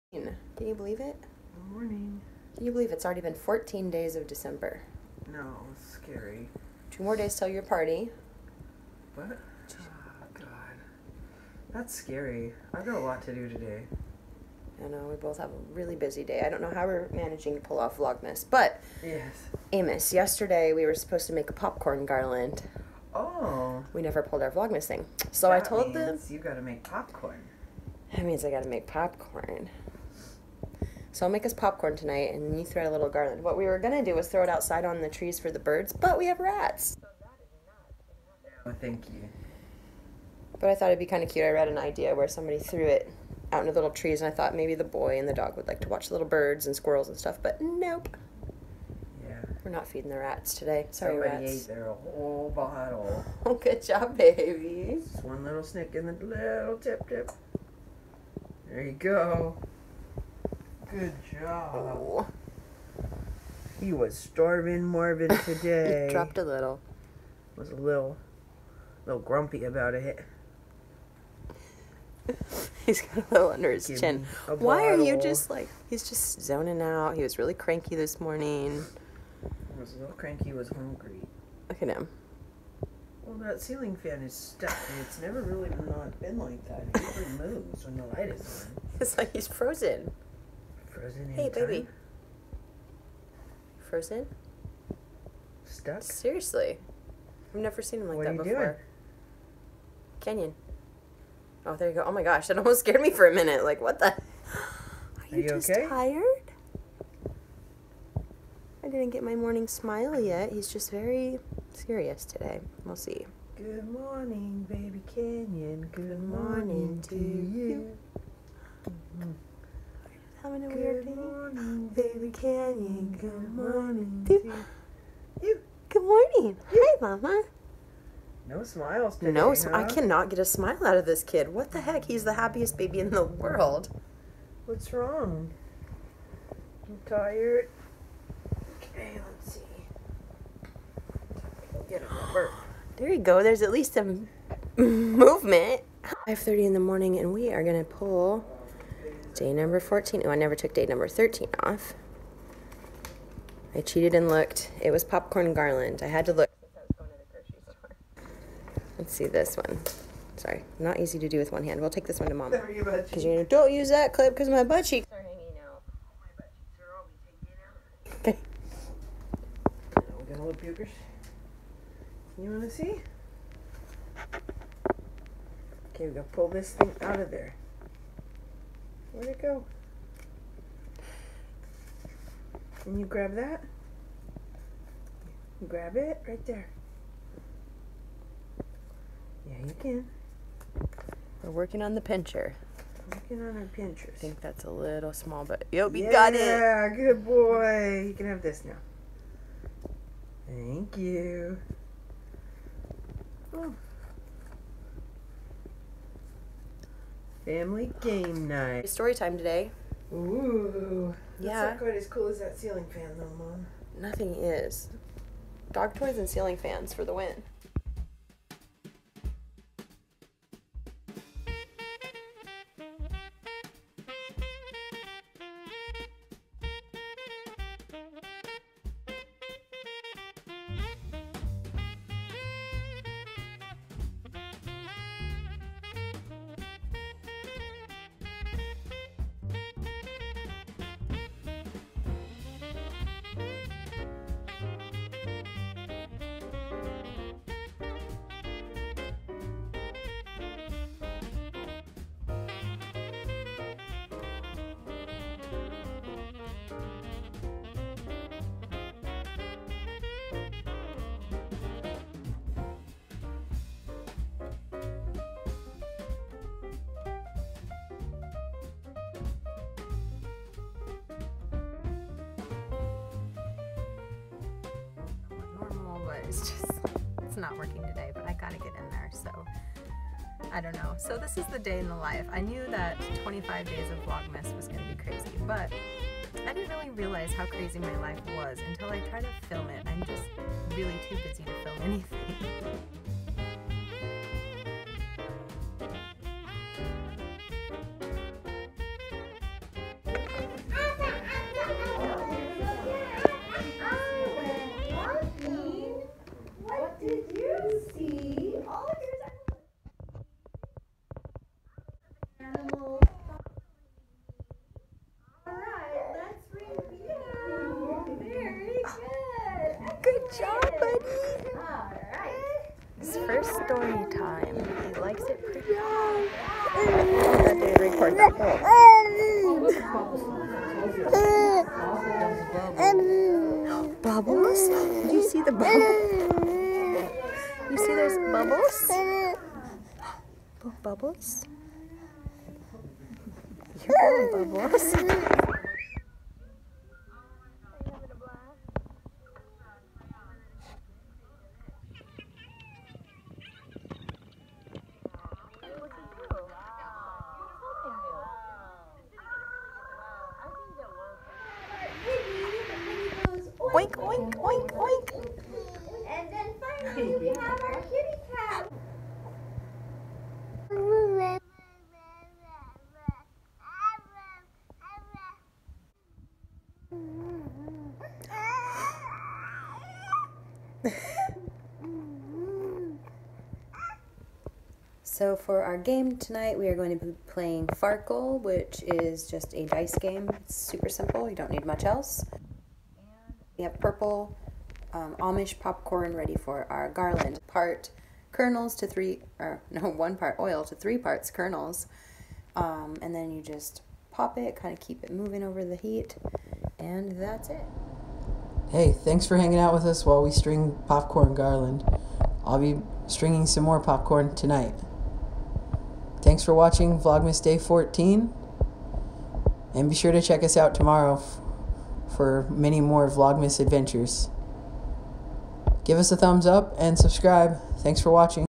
VLOGMAS | Day 14
Today we went to Baby Story Time at the library, caught up on Advent days 13 and 14 and now enjoying family game night - we will catch up with you tomorrow!

If you are new to our channel, check out the beginning of our infertility and TTC journey, beginning in 2013: